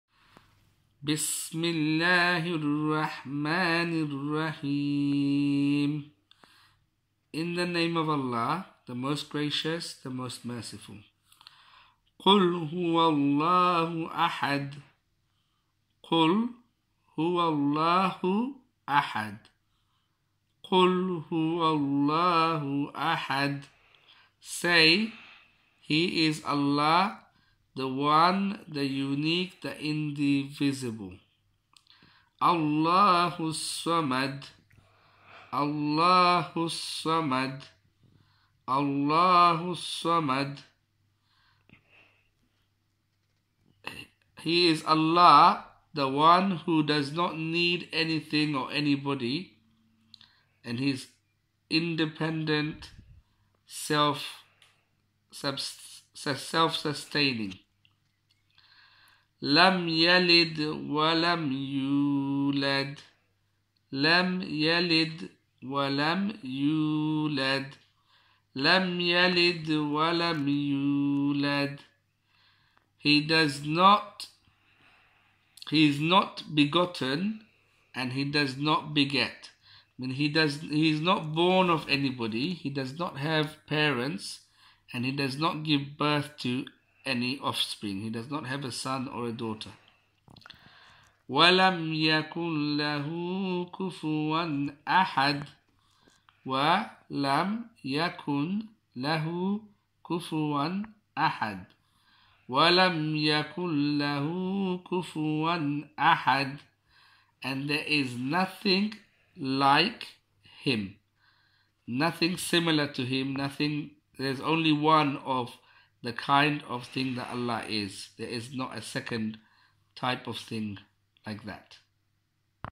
Surah Ikhlaas (Chapter 112: Sincerity) with meaning.
The third from last chapter of the Holy Quran. This short chapter is a description of God in a succinct way, such that no created being is mentioned in it ; only God.

It is considered to be the most powerful chapter in the Holy Quran and the most powerful political statement anyone can make. This is because the words contained in this chapter are a criterion to prove the superiority of the true God over all others that have been worshipped, whether imaginary or from the created beings.

The meaning of this verse translated by Abdullah Yusuf Ali is as follows:

1. Say: He is Allah, the One and Only;

2. Allah, the Eternal, Absolute;

3. He begets not, nor is He begotten;

4. And there is none like unto Him.